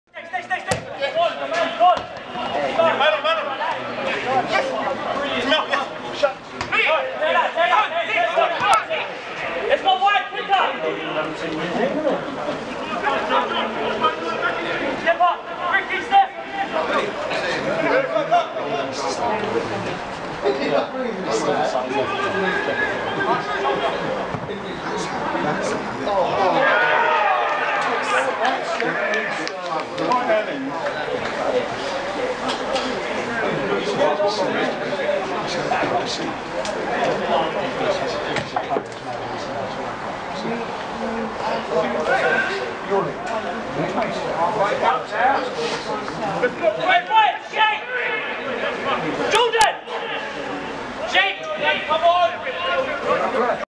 Newham 3-3 Barking. Essex Senior League. Wed10Feb2016
A dramatic end to this local derby at Tilbury's Chadfields, as title hopefuls Barking are made to pay for missed chances including a penalty, with bottom side Newham grabbing an injury time equaliser